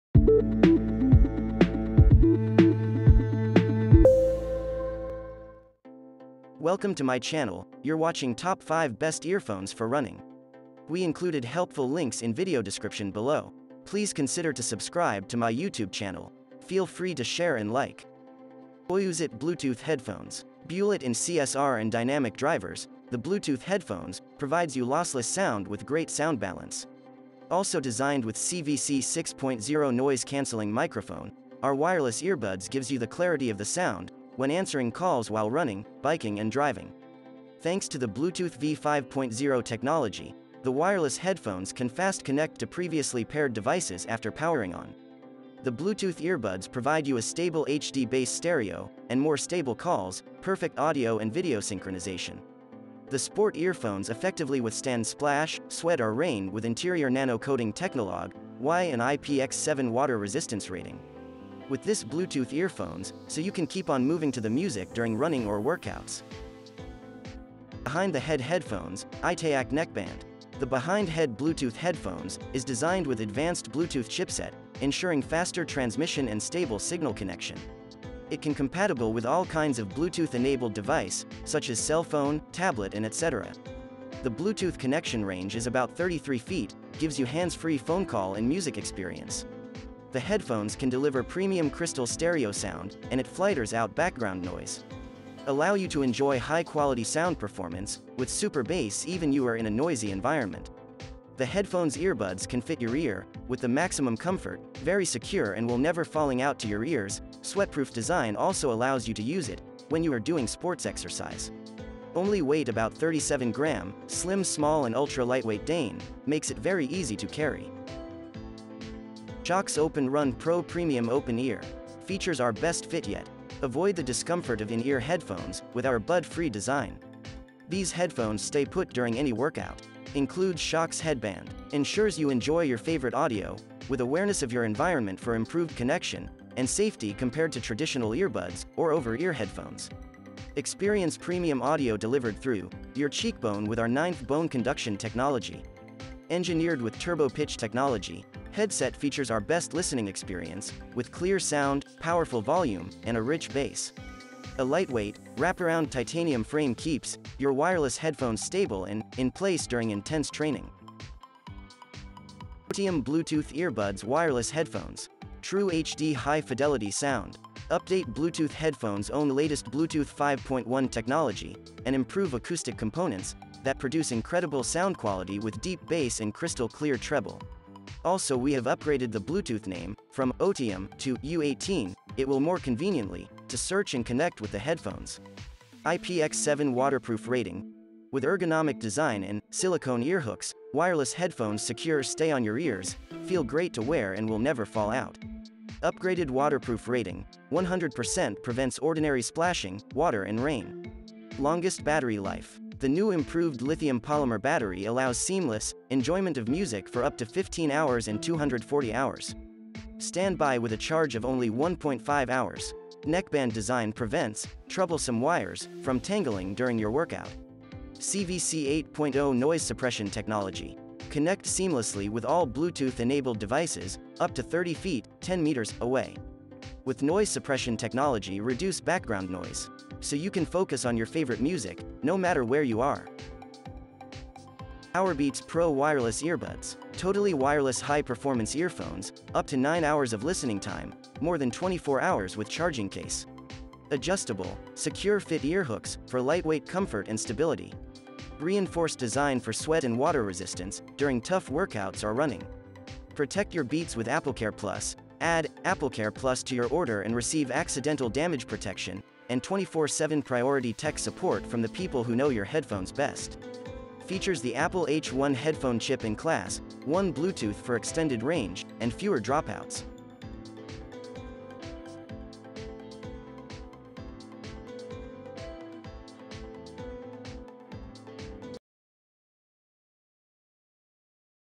Earphones For Running - Best Earphones For Running in 2022 [Top5]
The Best Earphones For Running is more geared to those who want their cans and buds to stay on their heads even while sprinting. So I look for some design element, a sport fin, wing or ear hook when it comes to earbuds, that offers some added security.

Here Are The Top 5 Best Earphones For Running :

1.Powerbeats Pro Wireless Earbuds

2.Otium Bluetooth Earbuds Wireless Headphones

3.Shokz OpenRun Pro Premium Open-Ear

4.Behind The Head Headphones, itayak Neckband

5.Poiuzet Bluetooth Headphones